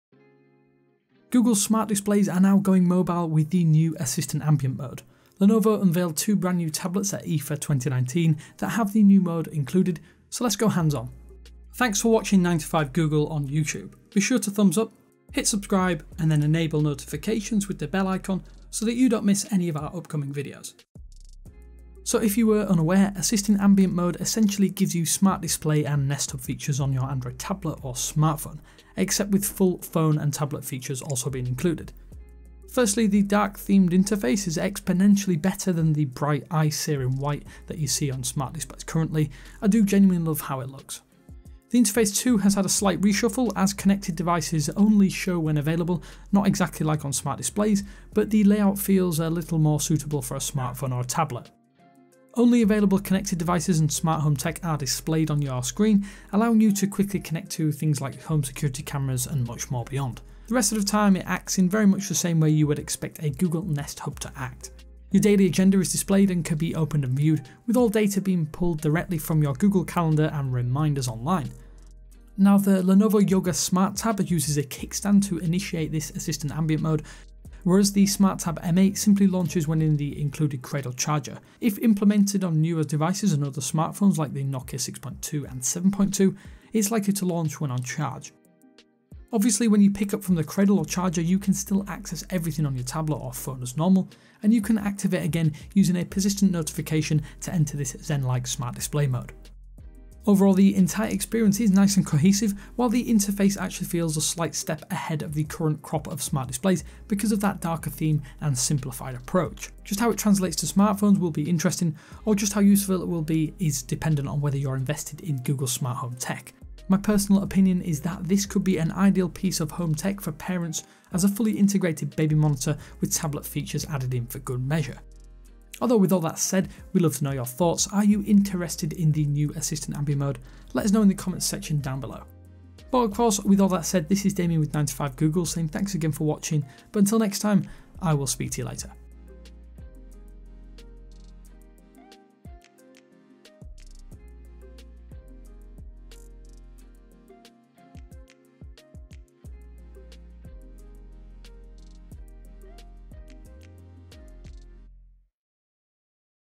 Assistant Ambient Mode hands-on: Portable Smart Display!
Smart Displays are coming to your Android smartphones and tablets. We go hands-on.

My 🎥🎙gear:
Panasonic Lumix GH5

Rode NTG-3